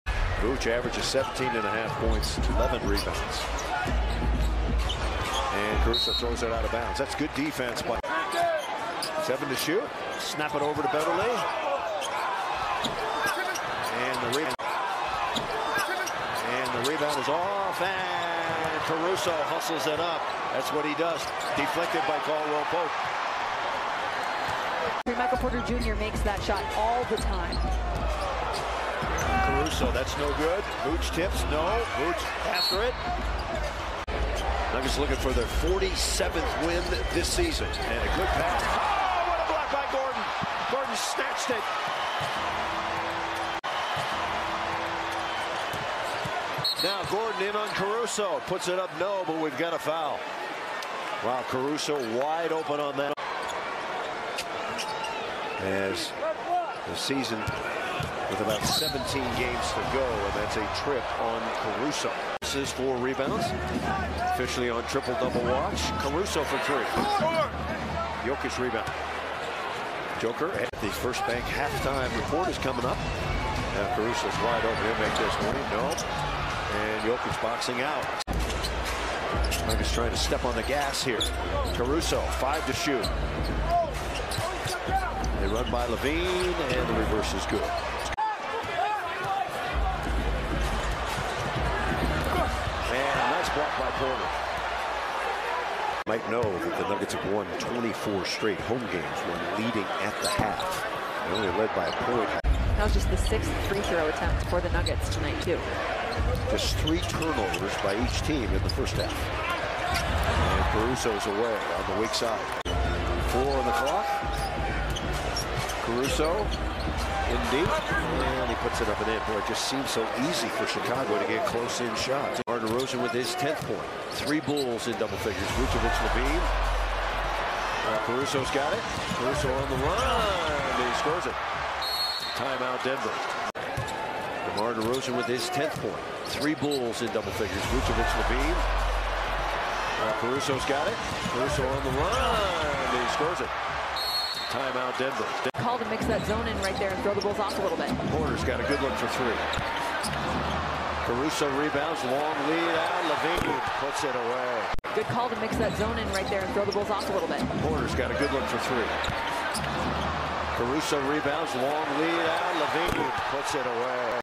Alex Caruso 6 PTS: All Possessions (2023-03-08)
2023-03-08 Denver Nuggets vs Chicago Bulls: Player Highlights Alex Caruso
    6 PTS, 3 REB, 2 AST, 1 STL.
        0 of 2 from 3.
        3 of 8 FGs in 17:38 minutes.